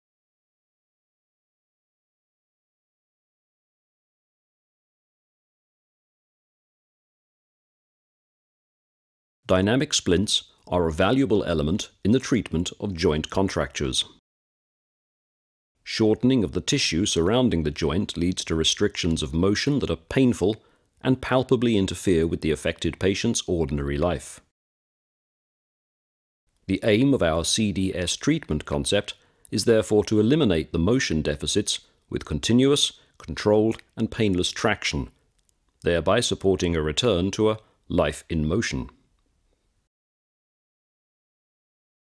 CDS Elbow Overview
The CDS® Elbow Brace Extension functions according to the CDS® principle and has been designed to treat an extension deficit of the elbow. The elbow contracture brace applies a dynamic low load prolonged stretch to stimulate growth in the contracted tissue. The adjustable redression range protects the tissues from overstretching and allows for individual extension up to -15°.